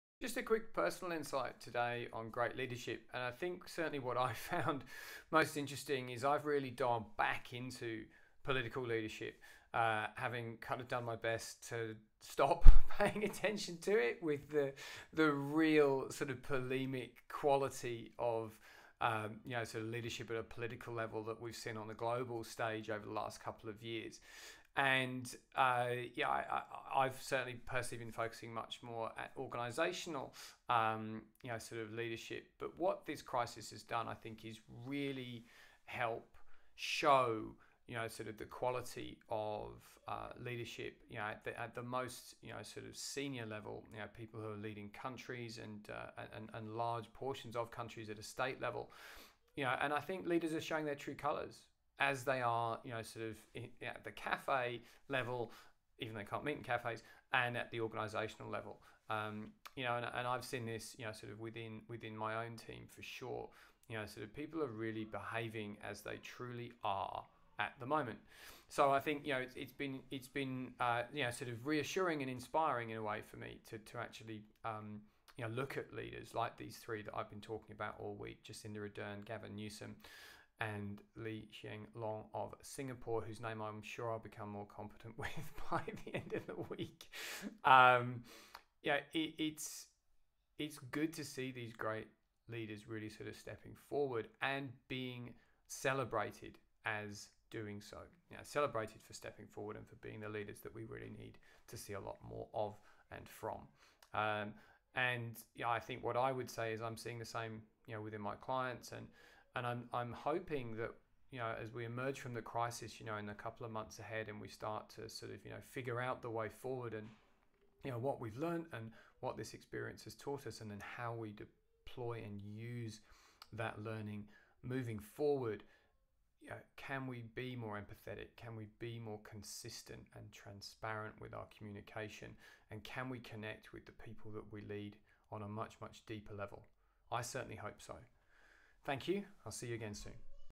Are you paying attention to politics again? I am, and here's why...
Today I'm sharing a personal insight on what I've leart from today's great leaders by dialing back in to the political landscape in recent months.

I must confess that in recent years I've stopped paying as much attention as I used to what the world's political leaders are saying and doing.

From Trump, to Brexit, multiple PM changes in Australia, the rise of nationalism to challenge globalisation, there's been so much noise and unhelpful argurement and speculation that frankly, I just stopped paying attention most of the time.

The run of calamity and crisis that we've endured this year, especially for me locally from fire to flood and now the Coronavirus crisis, I've re-embraced observing the political arena with one clear focus (leadership) and one question.  How do the great leaders at a political level respond to circumstances like these?

What I see is leaders showing their true colours. Showing who they are and how they lead in the full glare of scrutiny, stress and pressure. There is simply nowhere to hide with the pace and depth of the change that's being experienced today.

I find this refreshing, and it's allowed me to hone in quickly on the fine examples of leadership that I find inspiring, and to take reassurance from those that I believe are leading to make the world better who are really showing up right now.